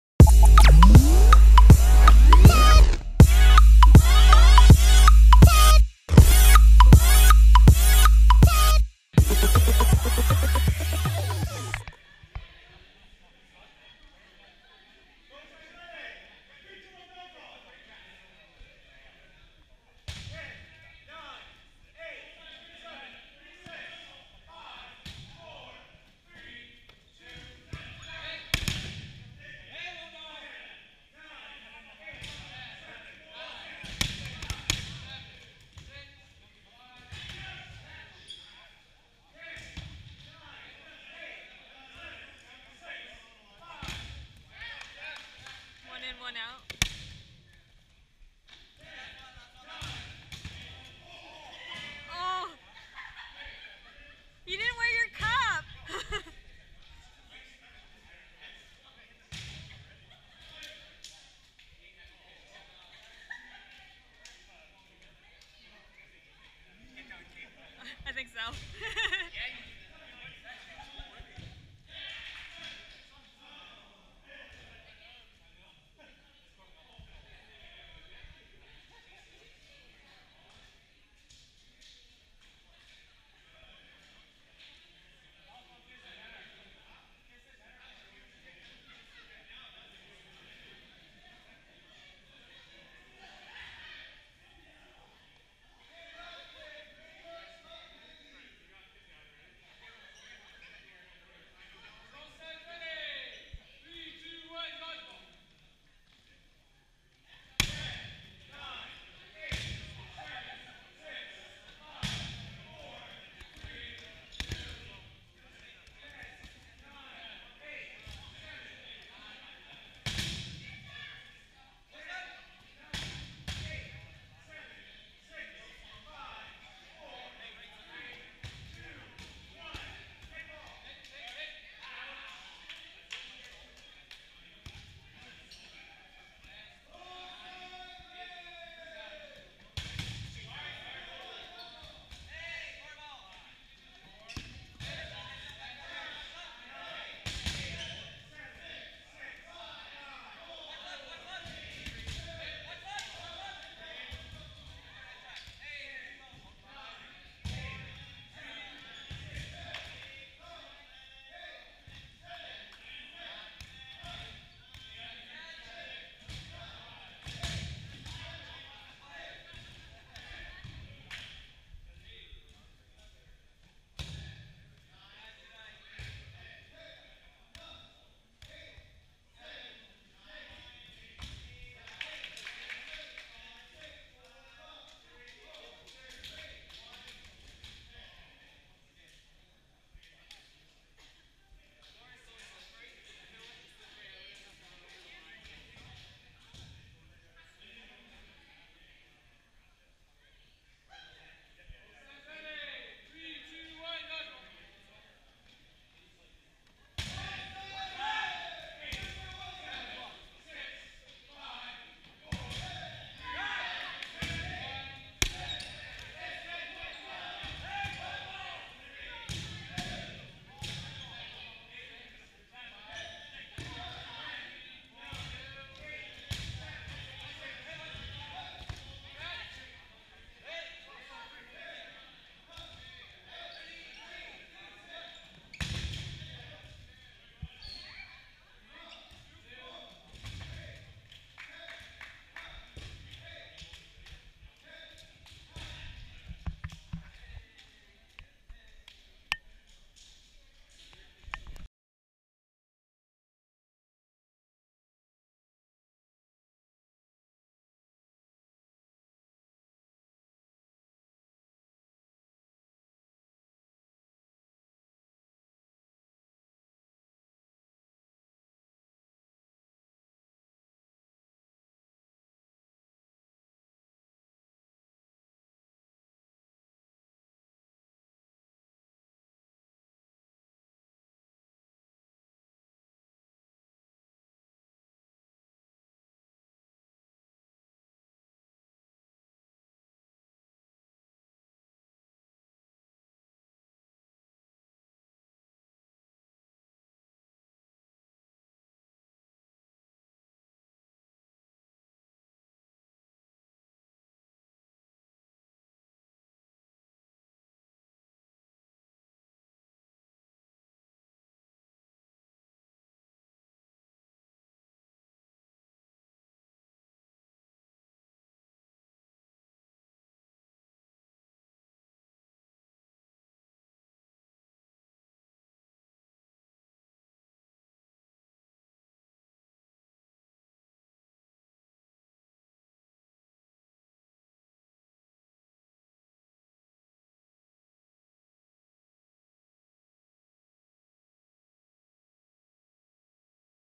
S7 W1 TUES SQUISHGATE VS BROCHACHOS
Surrey Dodgeball League

SEASON 7: WEEK 1- TUESDAY

squishgate vs Brochachos